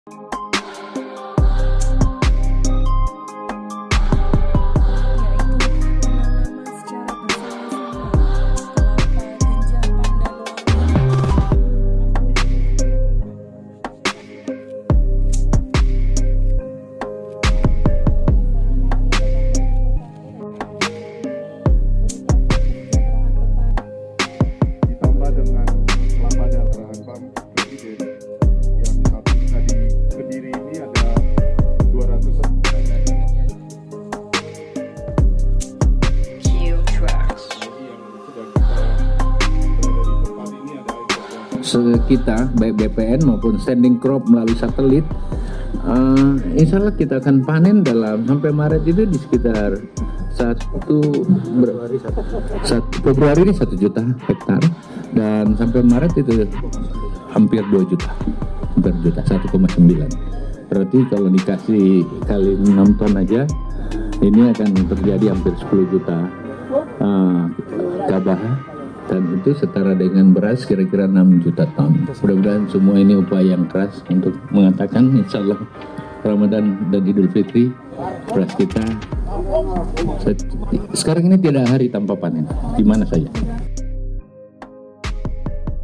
Menteri Pertanian Pastikan Stok Beras Aman Hingga Idul Fitri
KEDIRI, KOMPAS.TV - Menteri Pertanian, Syahrul Yasin Limpo, memastikan bahwa ketersediaan beras untuk Ramadan dan Idul Fitri aman. Hal itu diungkapkan Syahrul, saat berkunjung ke kabupaten Kediri, kamis siang.

Menurut Syahrul, hingga bulan Maret mendatang, 1,9 juta hektar lahan sawah akan panen. Dengan jumlah lahan tersebut, maka sedikit akan dihasilkan 10 ton gabah.

Artinya akan ada 6 juta ton beras yang dihasilkan oleh para petani dalam negeri, hingga bulan Maret mendatang. Data tersebut diperoleh kementan dari 3 sumber yang berbeda.

Dengan jumlah tersebut, Syahrul Yasin Limpo menjamin ketersediaan beras untuk Ramadan dan Idul Fitri aman.